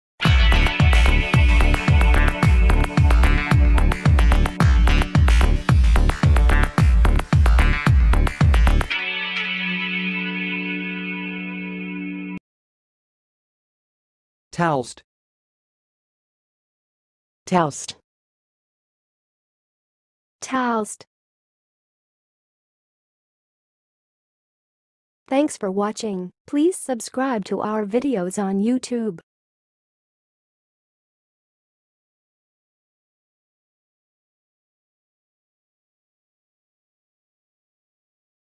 How to say "talced"! (High Quality Voices)
Watch in this video how to say and pronounce "talced"!